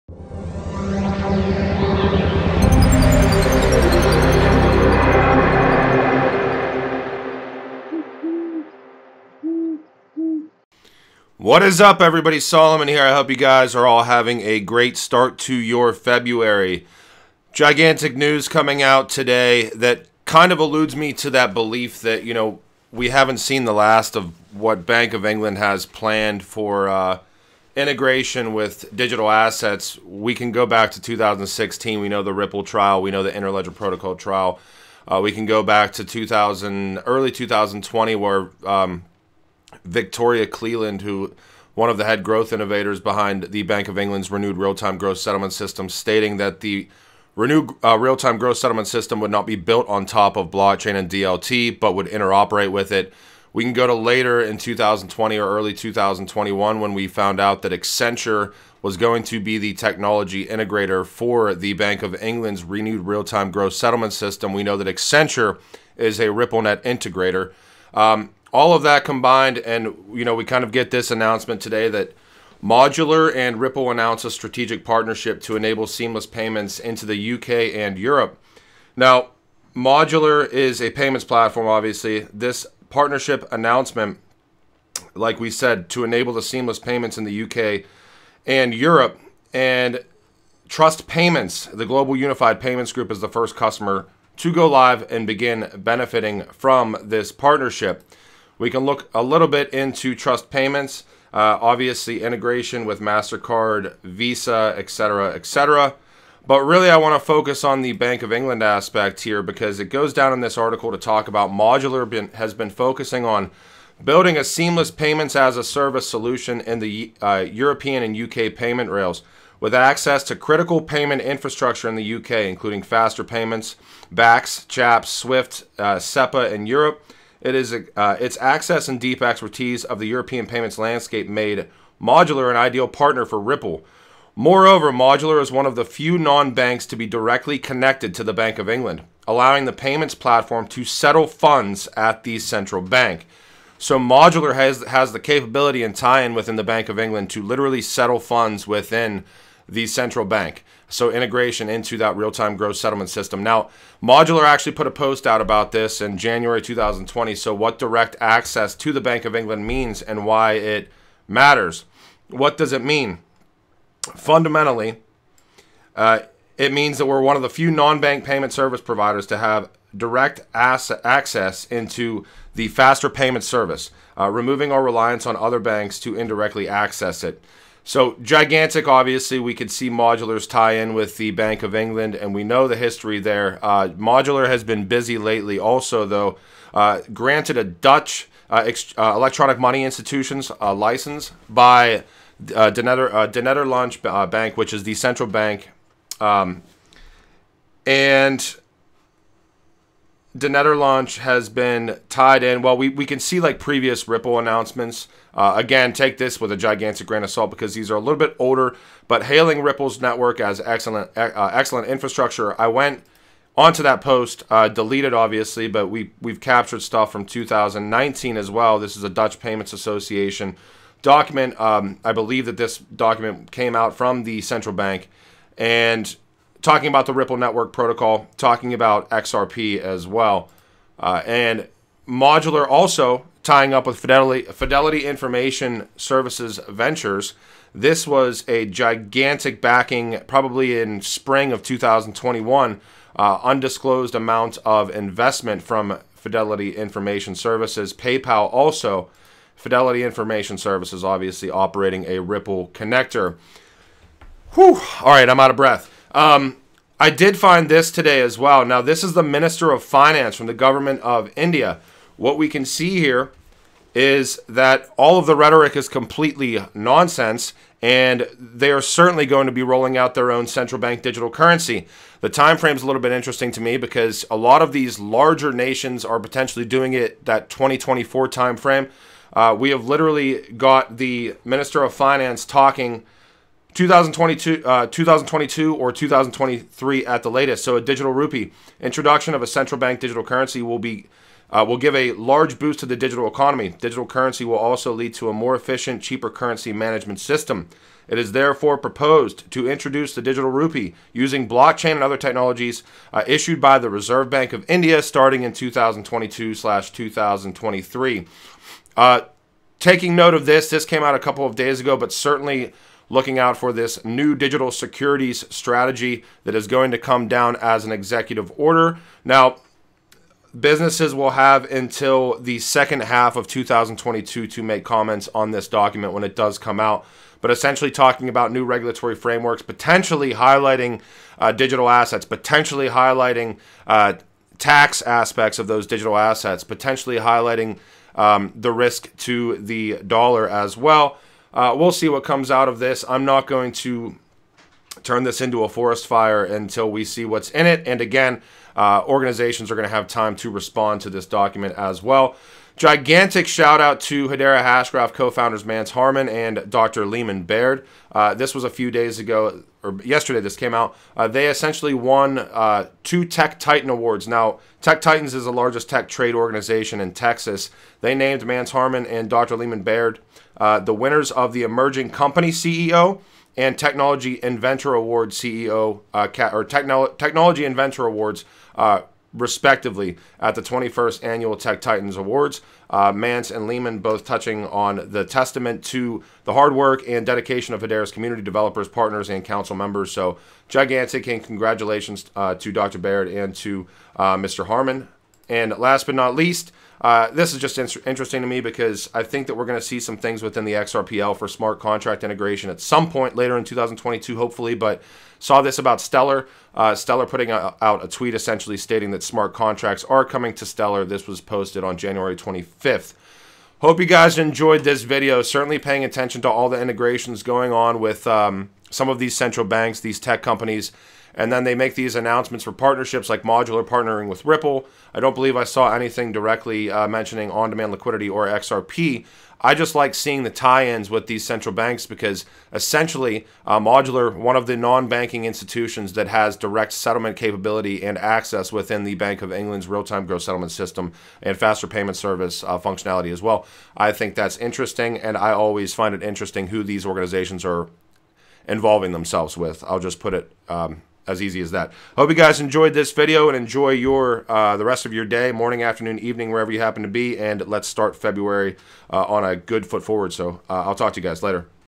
XRP/Ripple NEW PARTNERSHIP Accesses Bank of England Real Time Gross Settlement System
Interesting Moves Being Made
Also:
Executive Order on Crypto Incoming?
Hedera Founders Receive Tech Award